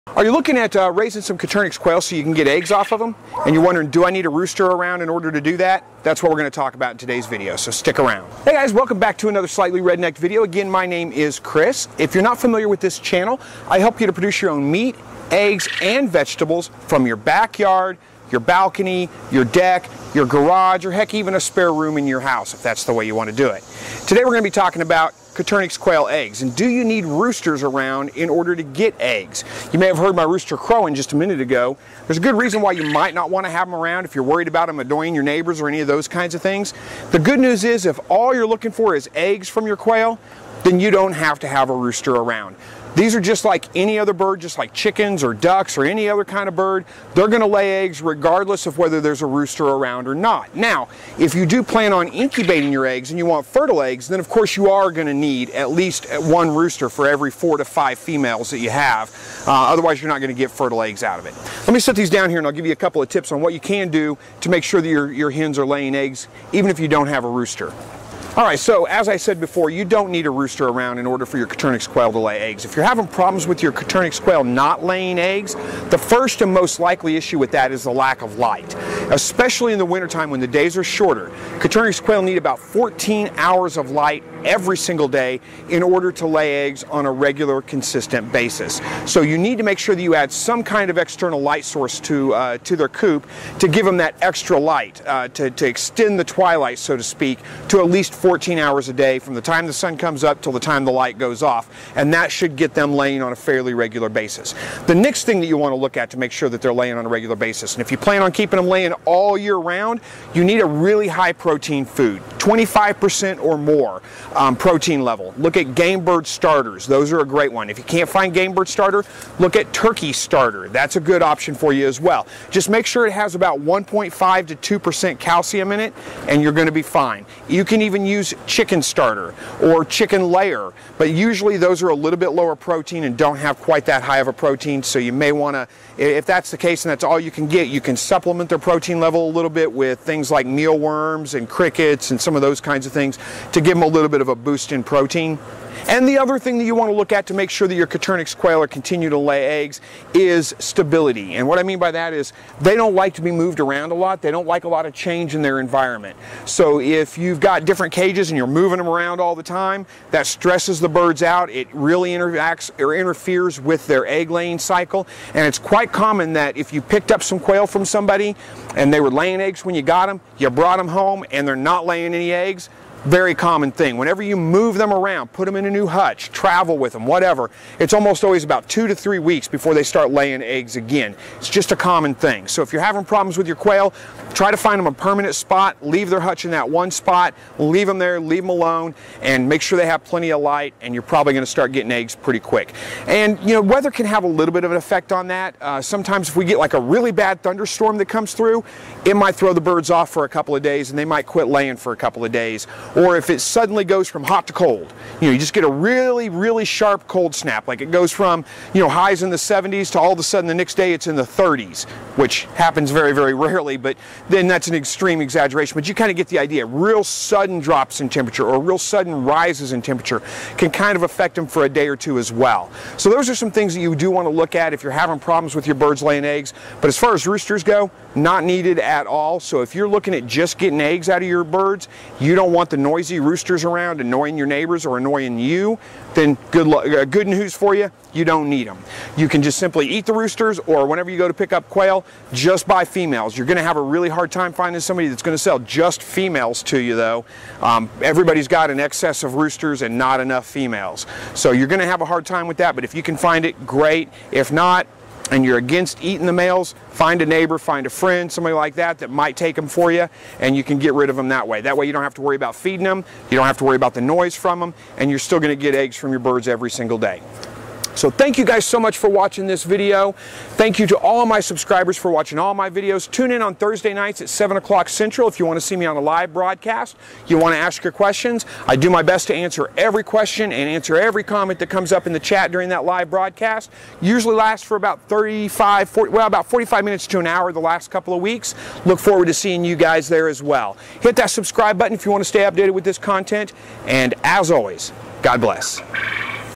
Will Coturnix Quail Lay Eggs Without a Rooster Around - The SR Quail Update 7-31-17
I have had a lot of people ask do you need a rooster for quail to lay eggs.  The quick answer is no you don't but in this video I go into a little more detail about what you do need in order to get your quail to lay eggs.